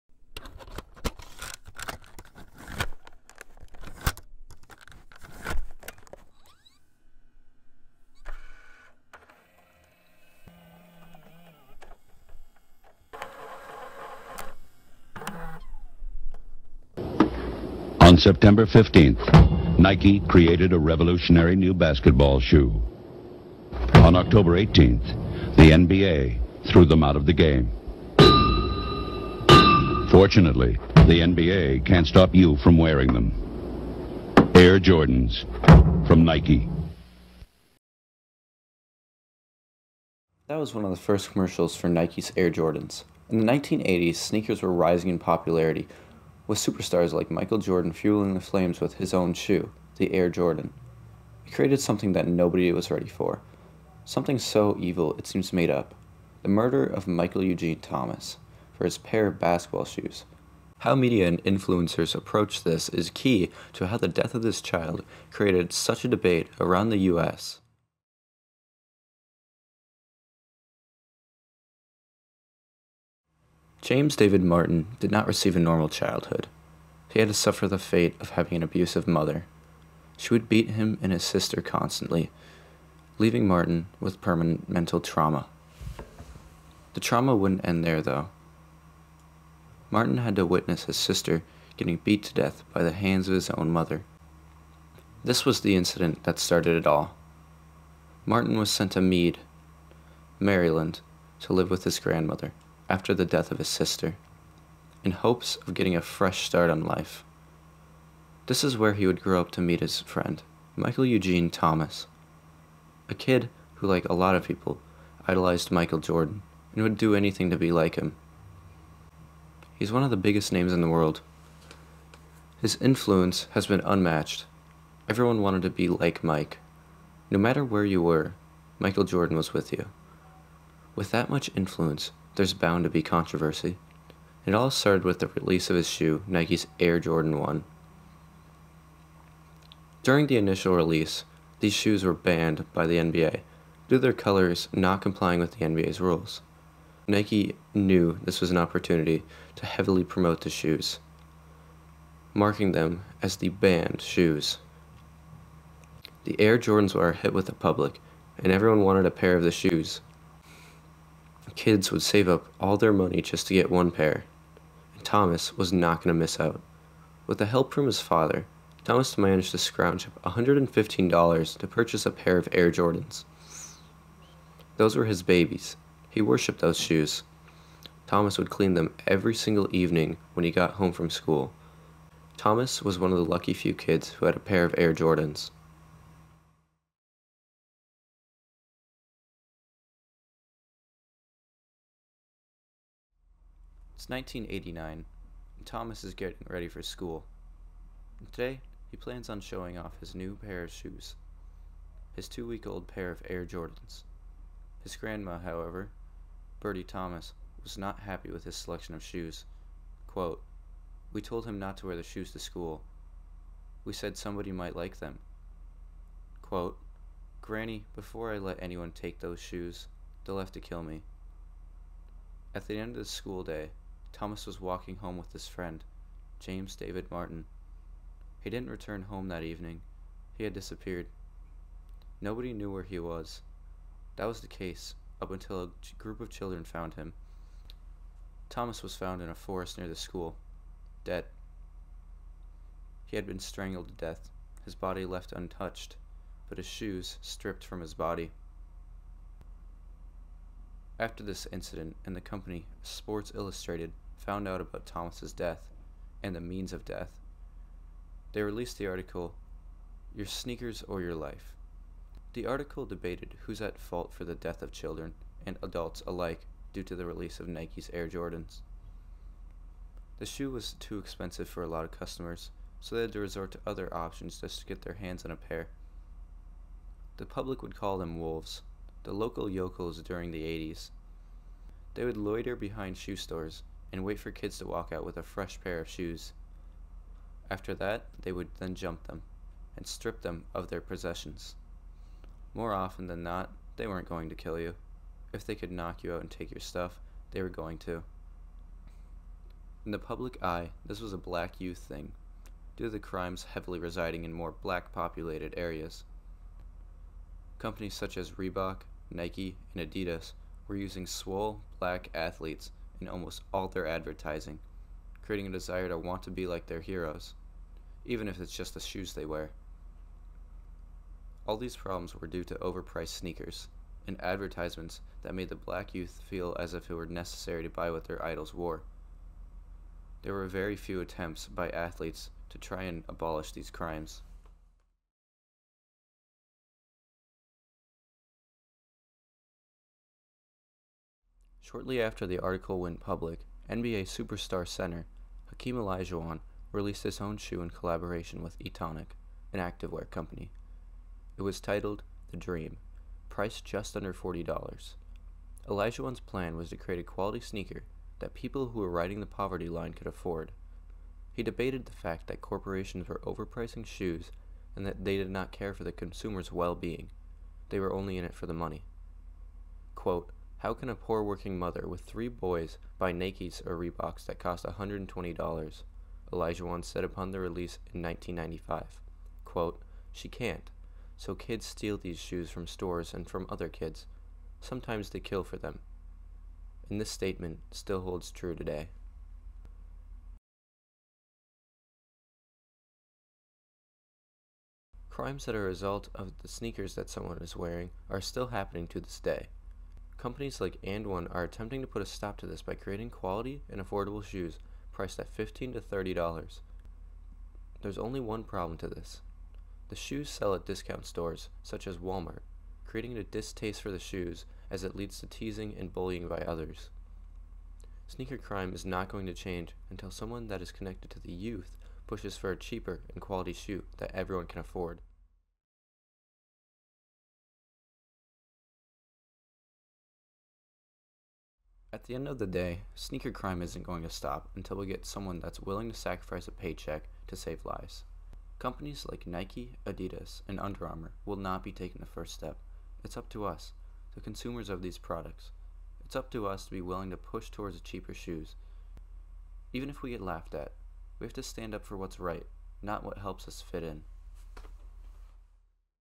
Killer Sneakers: Olajuwon’s Influence on the Shoe Craze Debate
Reide Strand, junior
Individual Documentary (Senior Division)
North Lakes Academy School Event